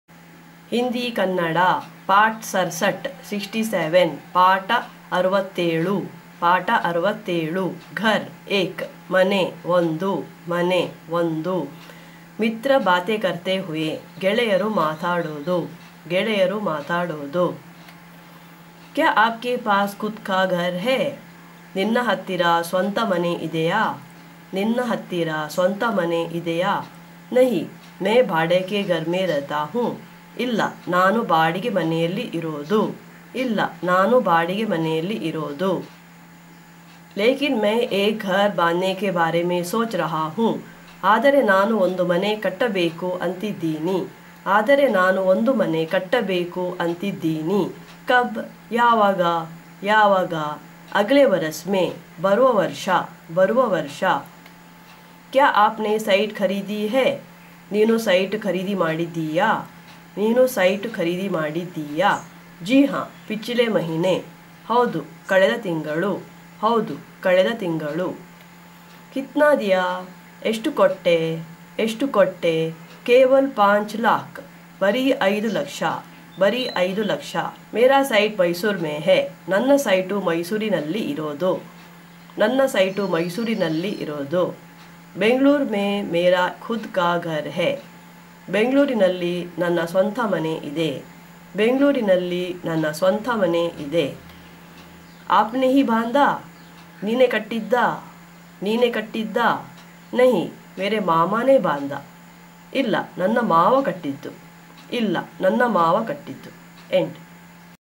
SPOKEN HINDI - KANNADA : LESSON 67 : GHAR - 1
Learn to speak Kannada through Hindi or Hindi through Kannada.